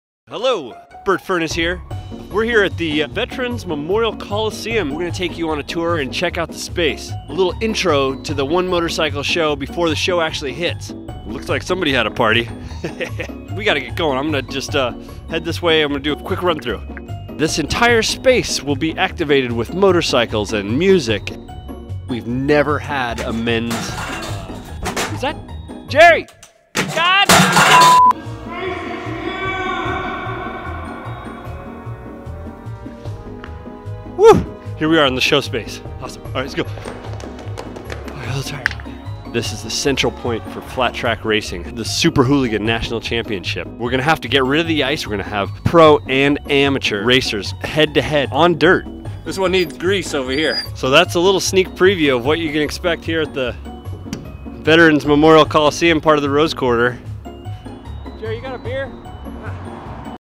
Burt Furnace: Veterans Memorial Coliseum Tour (The 1 Moto Show 2020)
The 11th Annual ONE MOTORCYCLE SHOW is at an all new venue! Come along on a quick video tour of the new show space with...*drumroll*...Burt Furnace! He'll show you all the in's and out's. The only downside was being was on the clock so Burt had to do a bit of running to get through this one!